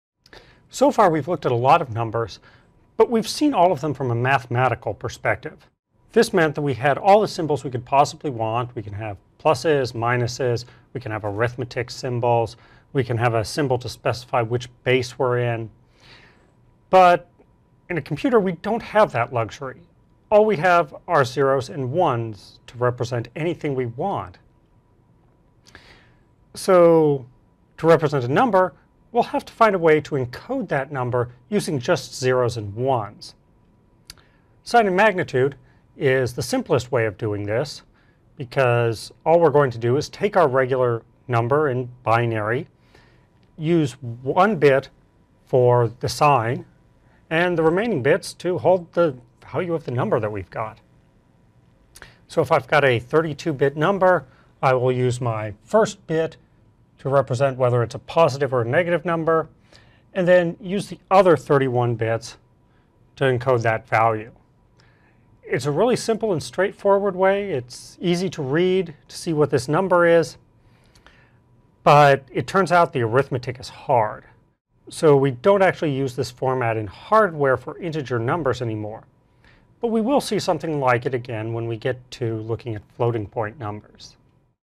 26. Sign & Magnitude Integer Format
Introduction to using the sign & magnitude format for representing integer numbers.